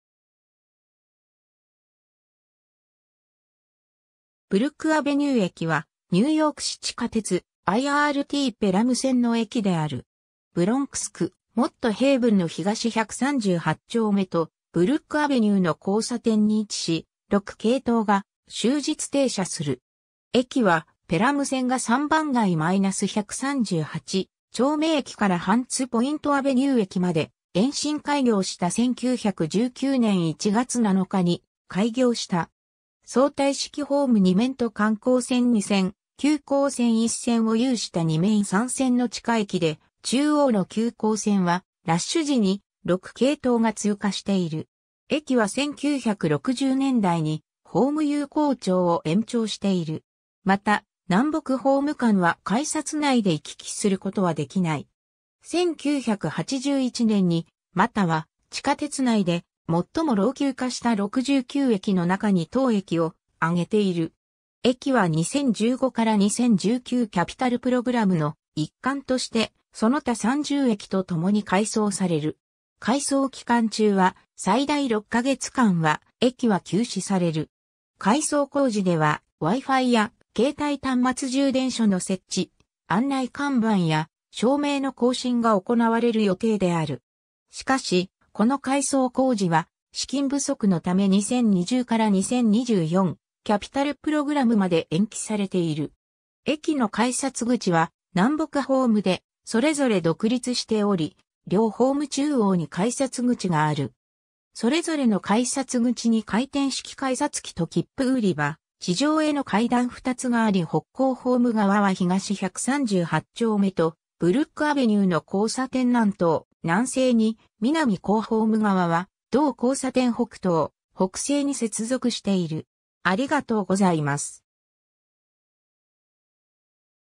ブルック・アベニュー駅
ブルック・アベニュー駅 (Brook Avenue) はニューヨーク市地下鉄IRTペラム線の駅である。
ブロンクス区モット・ヘイヴンの東138丁目とブルック・アベニューの交差点に位置し、6系統が終日停車する。
 駅はペラム線が3番街-138丁目駅からハンツ・ポイント・アベニュー駅まで延伸開業した1919年1月7日に開業した。
相対式ホーム2面と緩行線2線・急行線1線を有した2面3線の地下駅で、中央の急行線はラッシュ時に「6」系統が通過している。
 駅は1960年代にホーム有効長を延長している。
また、南北ホーム間は改札内で行き来することはできない。
 1981年にMTAは地下鉄内で最も老朽化した69駅の中に当駅をあげている。
駅は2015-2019 キャピタル・プログラムの一環としてその他30駅と共に改装される。
改装期間中は最大6ヶ月間は駅は休止される。
改装工事ではWi-Fiや携帯端末充電所の設置、案内看板や照明の更新が行われる予定である。
しかし、この改装工事は資金不足のため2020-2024 キャピタル・プログラムまで延期されている。
 駅の改札口は南北ホームでそれぞれ独立しており、両ホーム中央に改札口がある。
それぞれの改札口に回転式改札機ときっぷ売り場、地上への階段2つがあり北行ホーム側は東138丁目とブルック・アベニューの交差点南東・南西に、南行ホーム側は同交差点北東・北西に接続している。